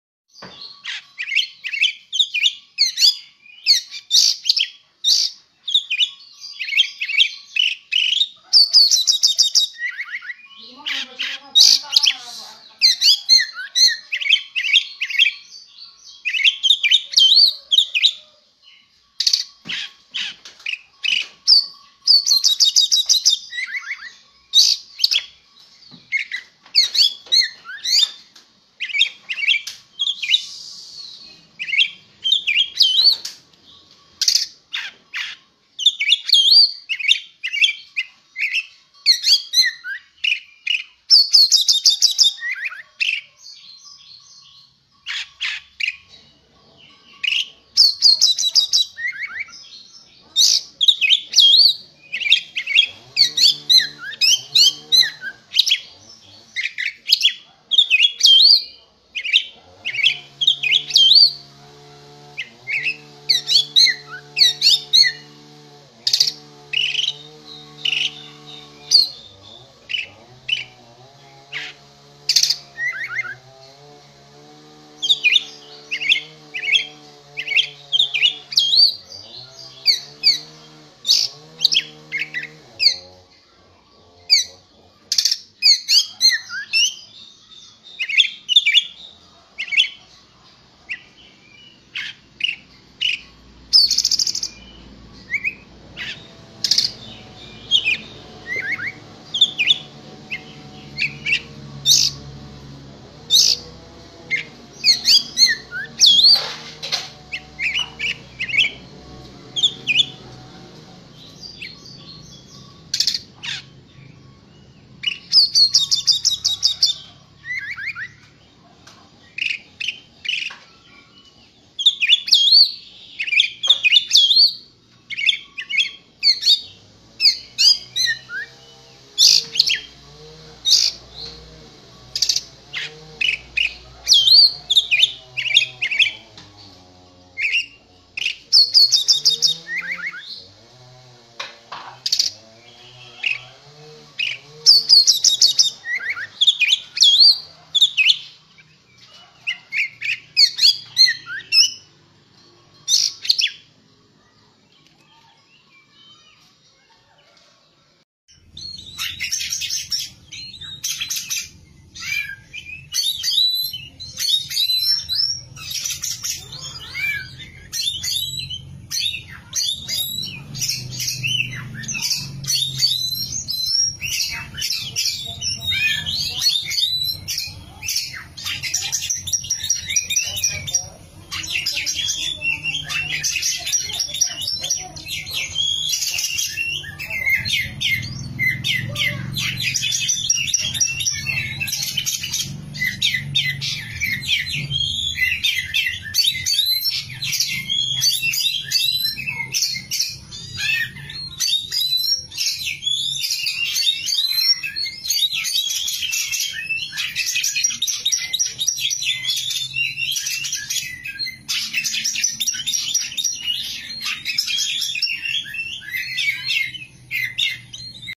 Ciri ciri KACER SIAP GANTANGAN....DI JAMIN PASTI NYAUT..
Kicau kacer gacor masteran